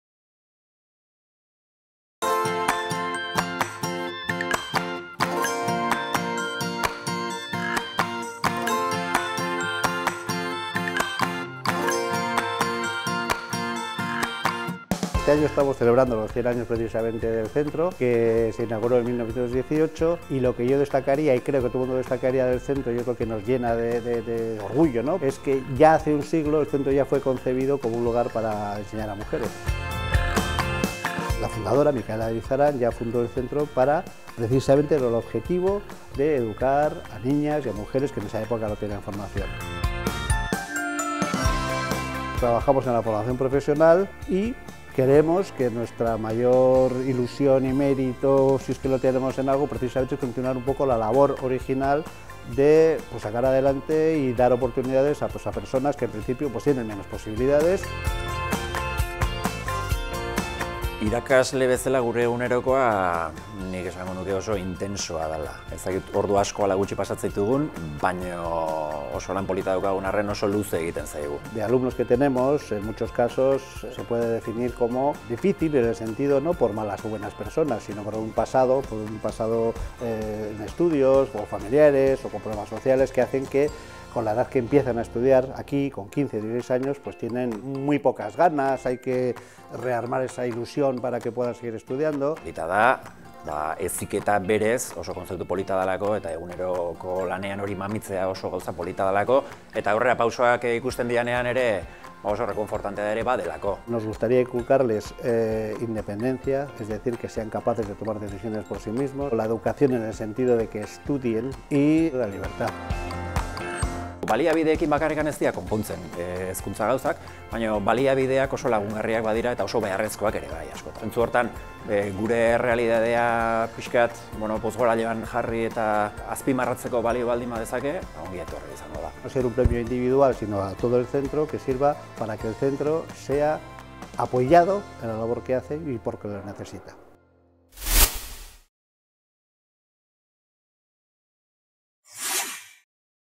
Medalla al Mérito Ciudadano 2019 Herritar Merezimenduaren Domina: Elizaran ikastetxea
Elizaran ikastetxea: bete berri dituen 100 urteotan milaka donostiarren lanbide heziketan eta bereziki, emakumearen sustapen eta laneratzean aitzindari izateagatik. elizaran.com/

Elizaran ikastetxea: por sus 100 años al servicio de la educación y la formación de miles de donostiarras y ser un centro pionero en la integración y promoción laboral de la mujer. elizaran.com/